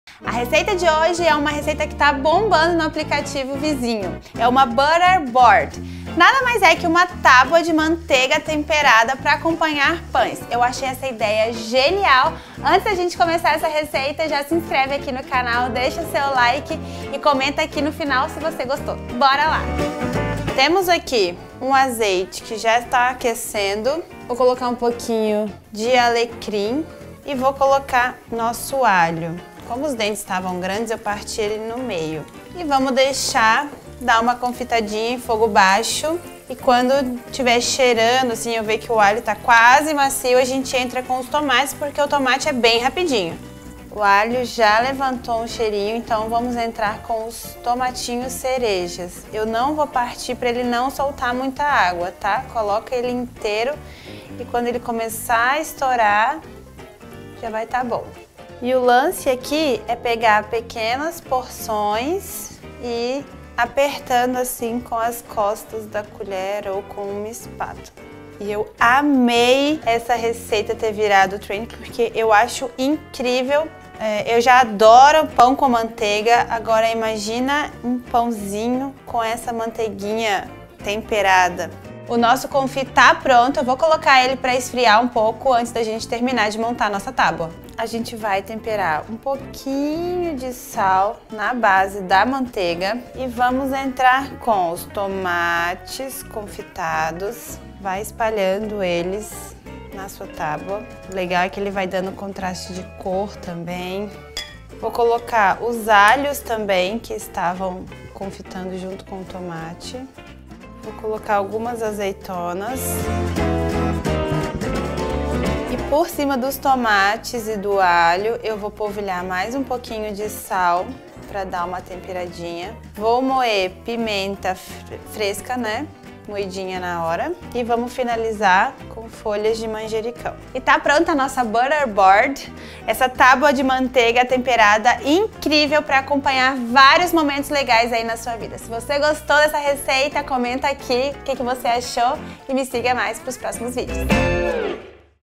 BUTTER BOARD
Hoje eu vou te ensinar a fazer a Butter Board, uma receita que está viralizada nas redes vizinhas!

Ingredientes:

200gramas de manteiga
100 gramas de tomates cereja
10 dentes de alho
100ml de azeite de oliva
sal
primenta do reino
folhas de manjericão
ramos de alecrim
ramos de tomilho

Equipe:

Produtora:  @Ópera de Vidro
Direção:  @Donnovan Ferreira
Assistente de Direção:  @Bruna Boita Meoti

Estratégia e Marketing:
Thauane Rizzi
Ingrid Santin

Cozinha | Doces | Chef | Cheff | Masterchef | Chapecó | Receita | Praticidade | Simples | Receita Rápida | Mineira | Minas Gerais | Culinária